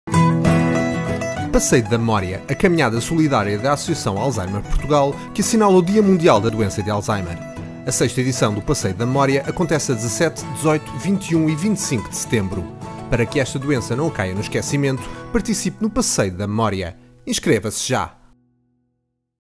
Passeio da Memória 2016
Passeio da Memória: A caminhada solidária da Associação Alzheimer Portugal, que assinala o Dia Mundial da Doença de Alzheimer.

17 de Setembro de 2016: Beja
18 de Setembro de 2016: Aveiro • Barreiro • Braga • Campo Maior • Fafe • Funchal • Matosinhos • Oeiras • Portimão • Viana do Castelo • Viseu
21 de Setembro de 2016: Pombal
25 de Setembro de 2016: Penafiel • Covilhã

Inscrições
Donativo Mínimo de 5,00€ por participante

‪#‎passeiodamemoria‬ ‪#‎diamundialdadoençadealzheimer‬ ‪#‎alzheimer‬ ‪#‎demencia‬ ‪#‎alzheimerportugal‬ ‪#‎aveiro‬ ‪#‎barreiro‬ ‪#‎beja‬ ‪#‎braga‬ ‪#‎campomaior‬ ‪#‎covilhã‬ ‪#‎fafe‬ ‪#‎funchal‬ ‪#‎matosinhos‬ ‪#‎oeiras‬ ‪#‎penafiel‬ ‪#‎pombal‬ ‪#‎portimão‬ ‪#‎vianadocastelo‬ ‪#‎viseu‬